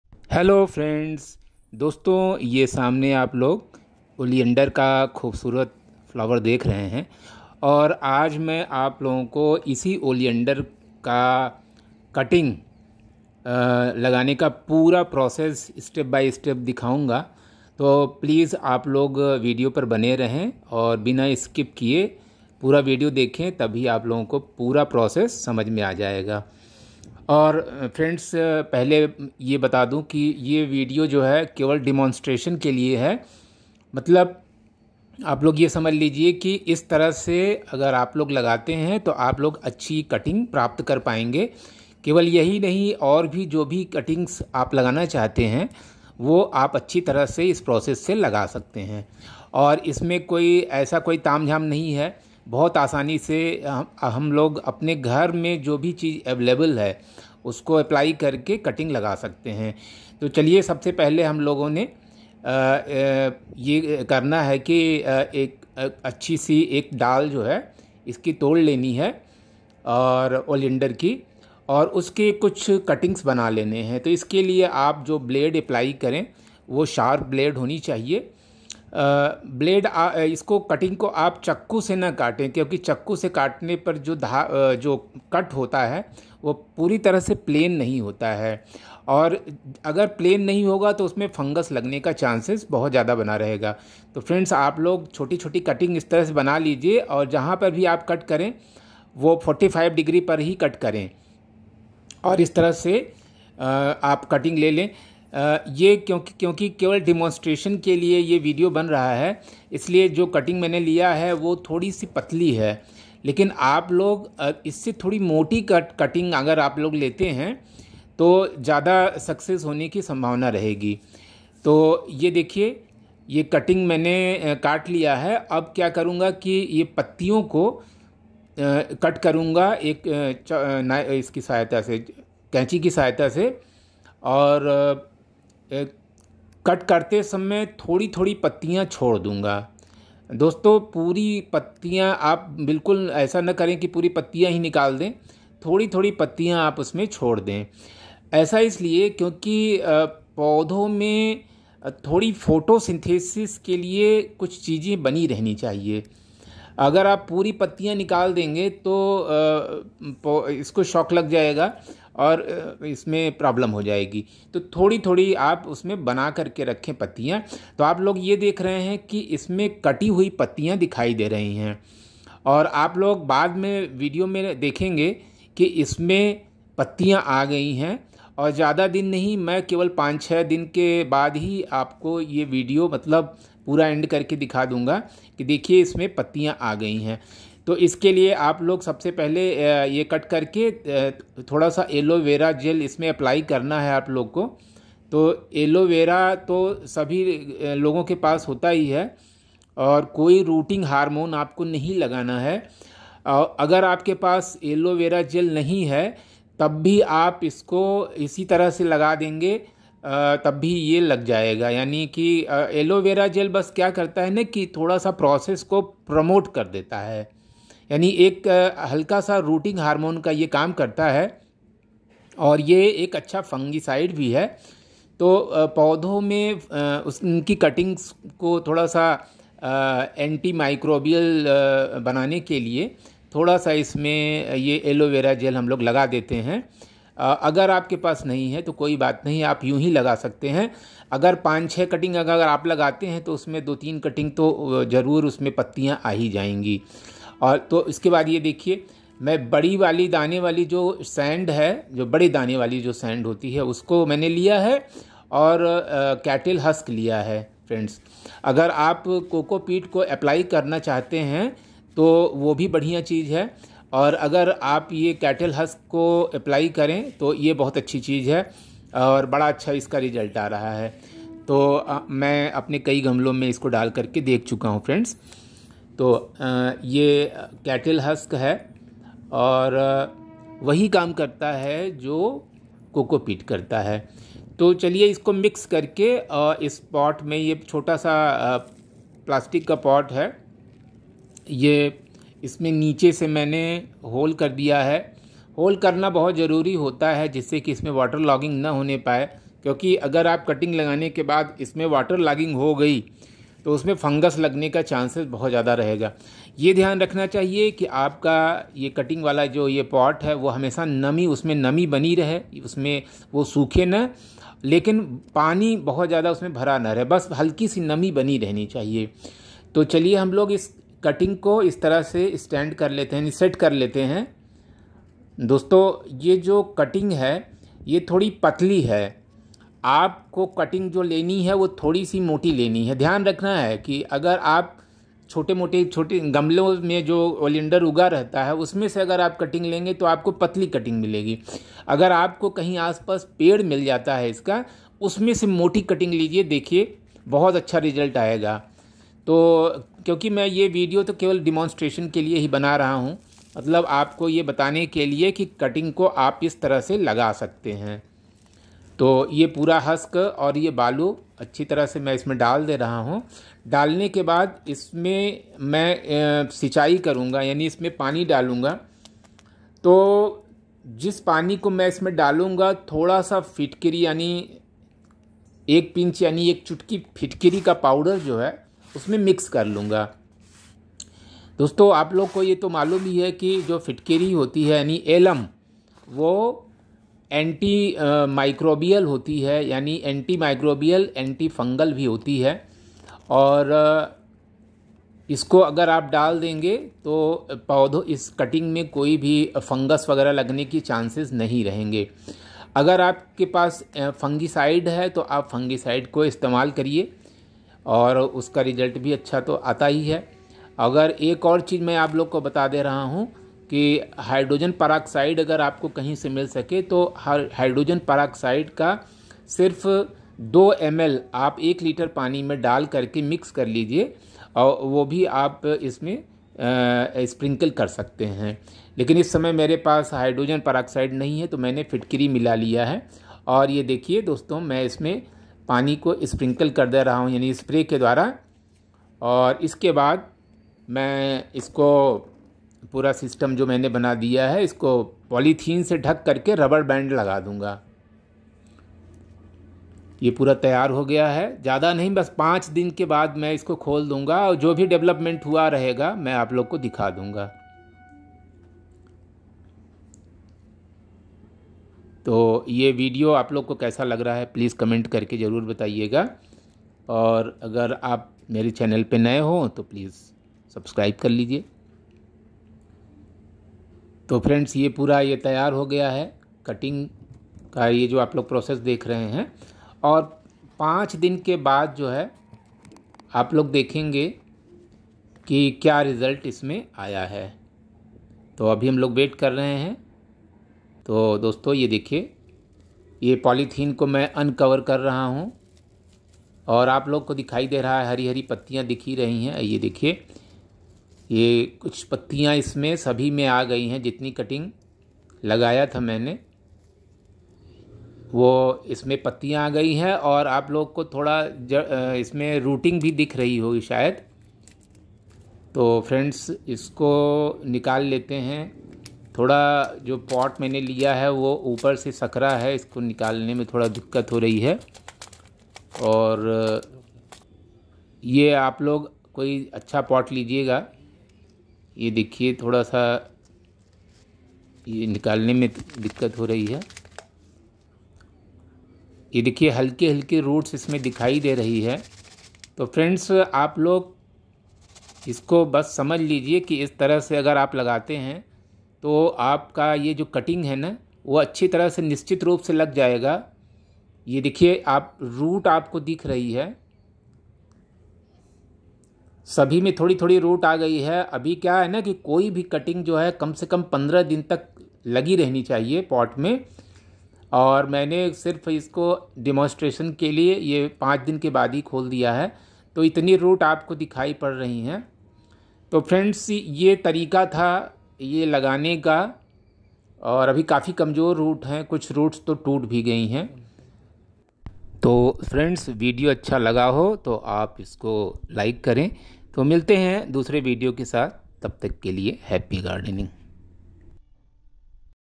बहुत आसानी से लगा लें ओलियंडर की कटिंग/Grow Oleander cuttings so easily!youtube
Friends!

ओलियंडर की कटिंग को हम बहुत आसानी से लगा सकते है। जिन पौधों में किट लगाने पर दूध जैसा sap निकलता है उनकी कटिंग्स आसानी से लग जाती हैं ।

वीडियो देखें!